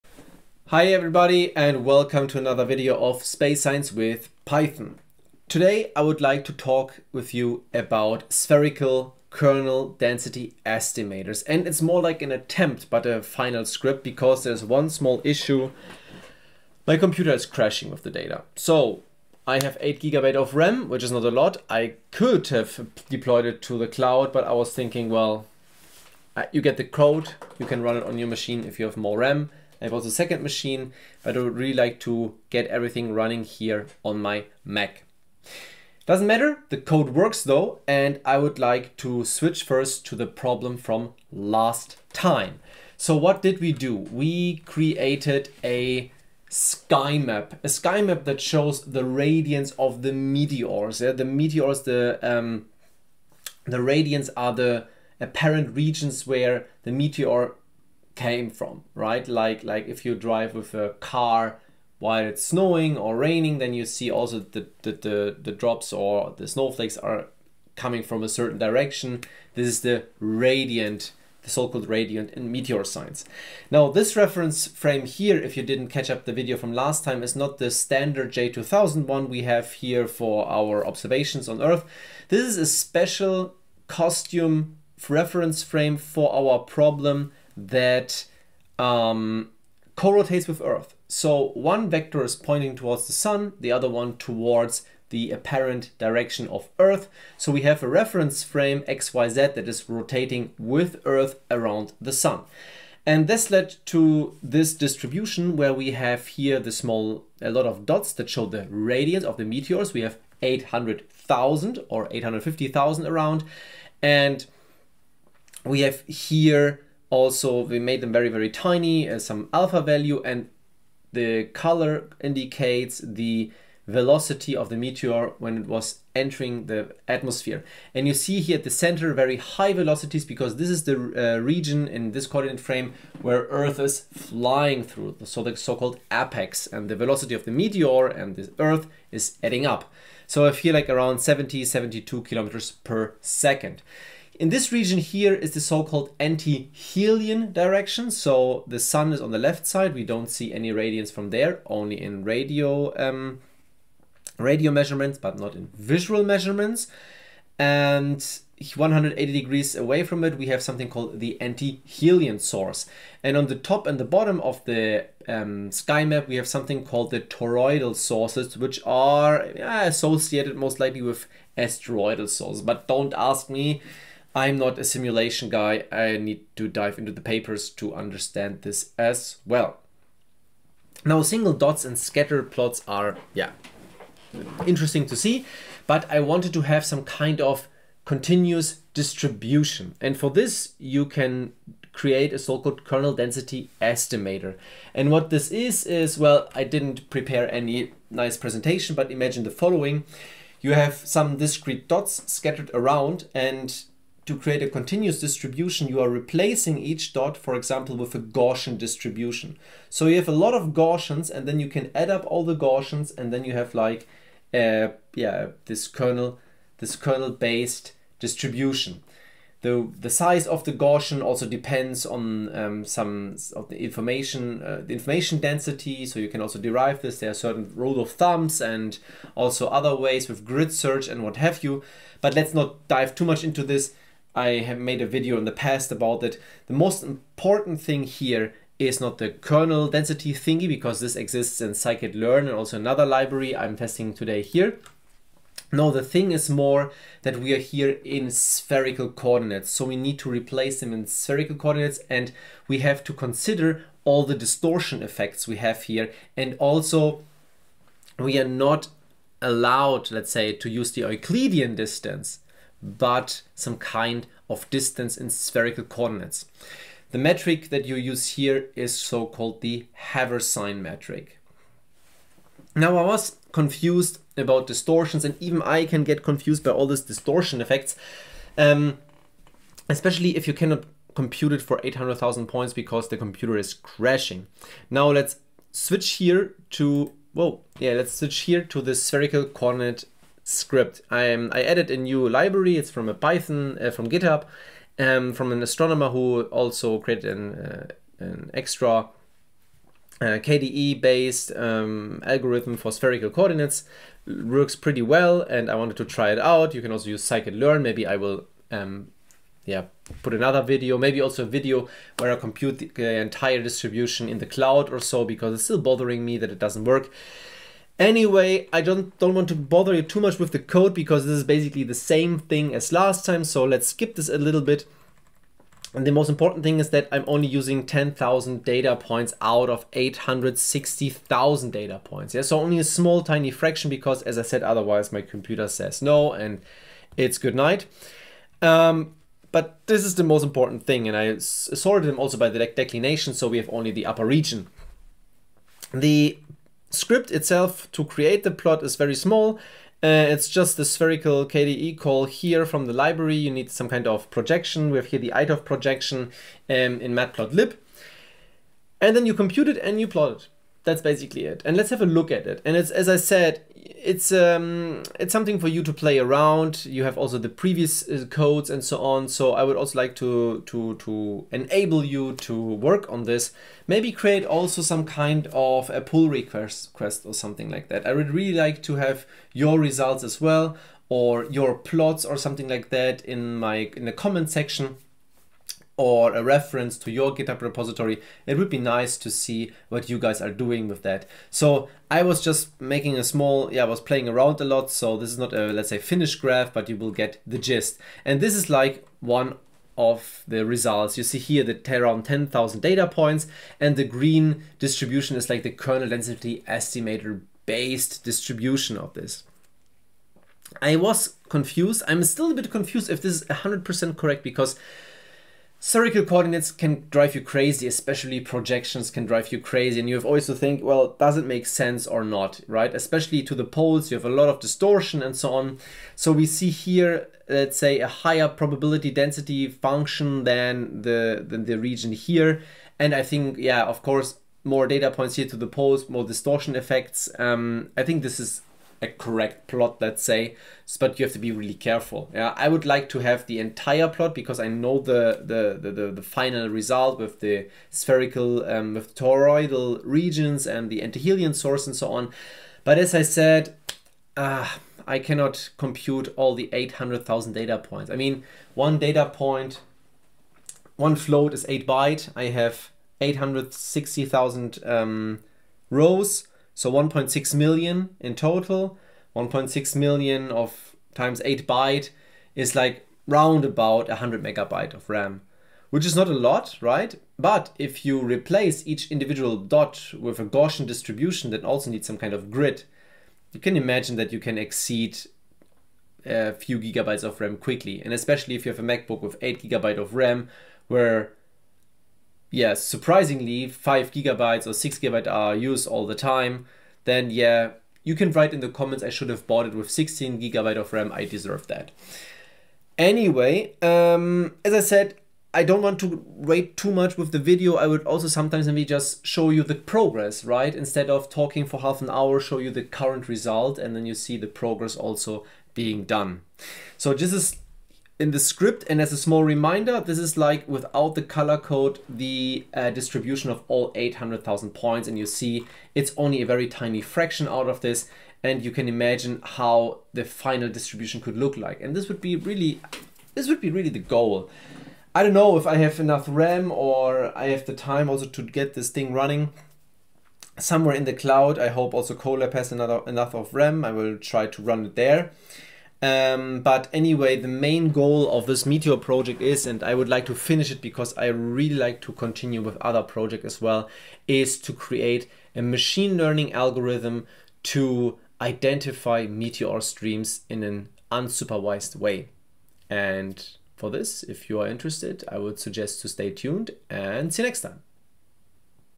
Space Science with Python - Meteor Science (7): Spherical KDE (attempt)
Long story short of today's session: Send RAM support please!

There is a lot to do and to learn and I hope you will join the journey.

Or drop a comment!
Talk to you later,
Thomas